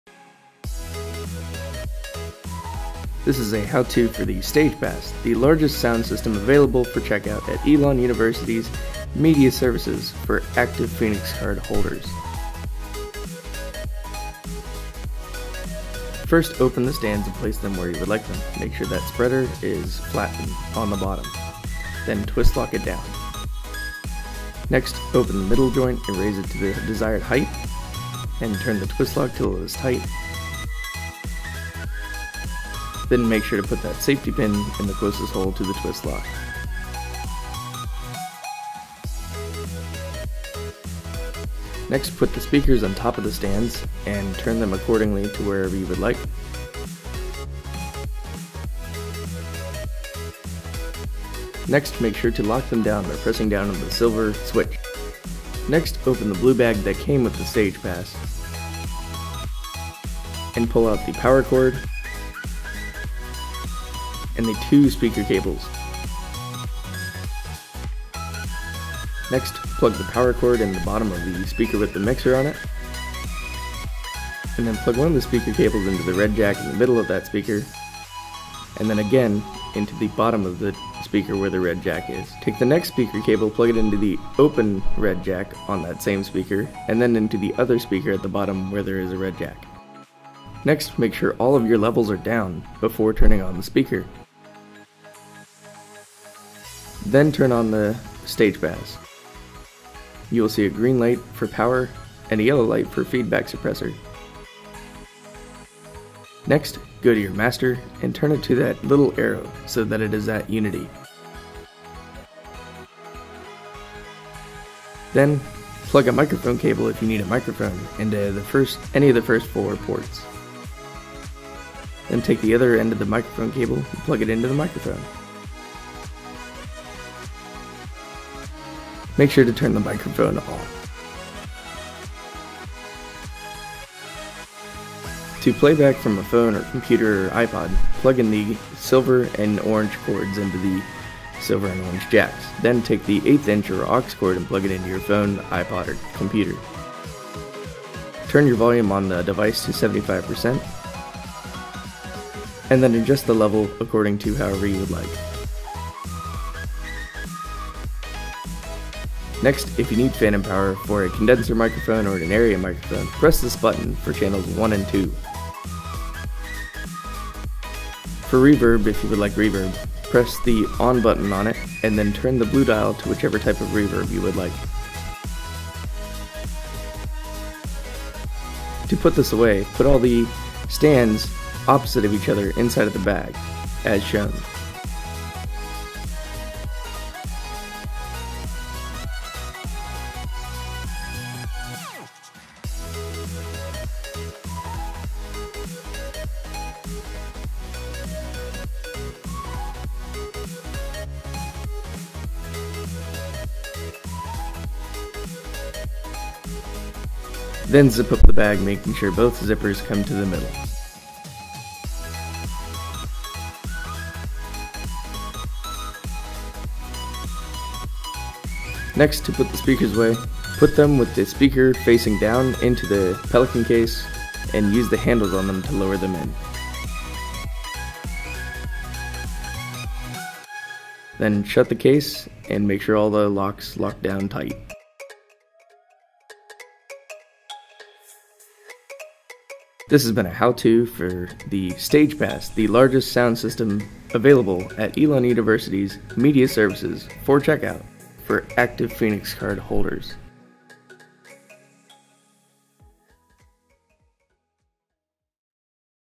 Basic How To: Yamaha Stage PA
This is a basic tutorial of how to use the Yamaha Stage PA.

This equipment is available for active Phoenix Card holders at Elon University's Media Services

Video Cred: Joshua Scovern
Voice: Joshua Scovern
Editing: Joshua Scovern